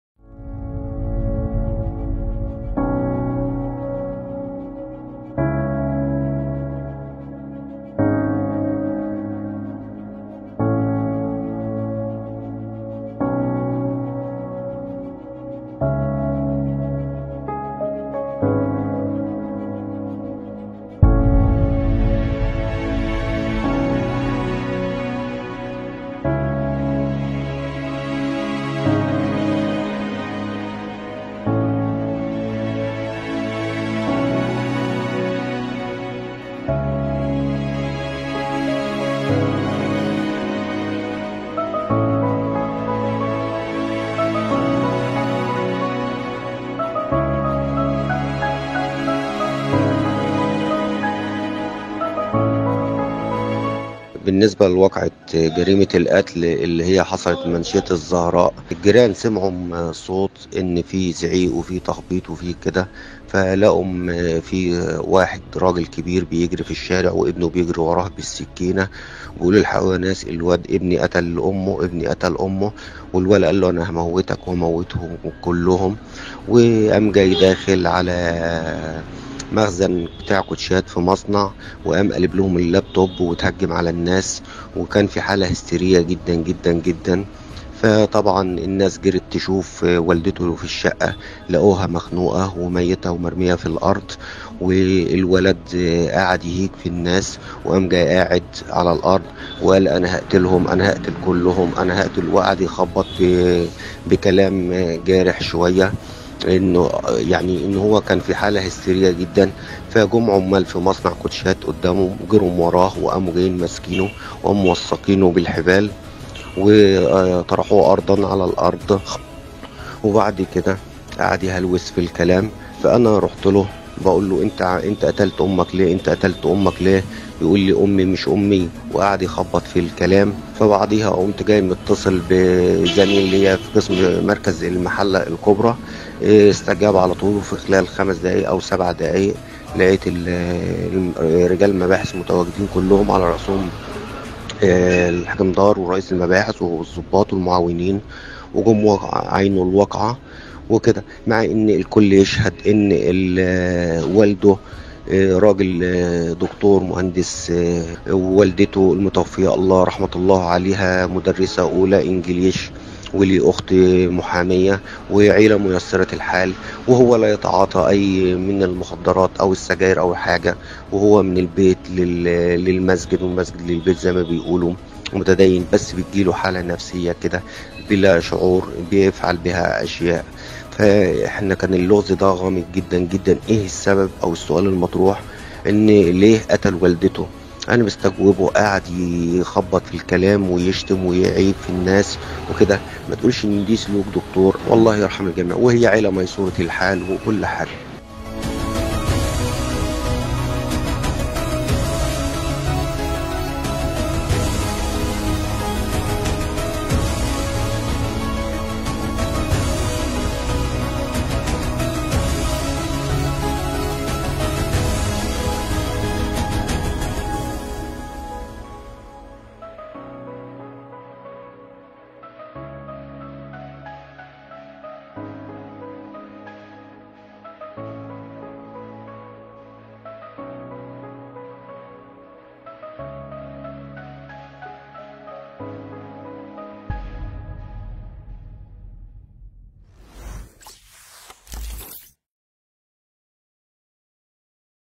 صيدلي ينهي حياة والده
ويحاول قتل امه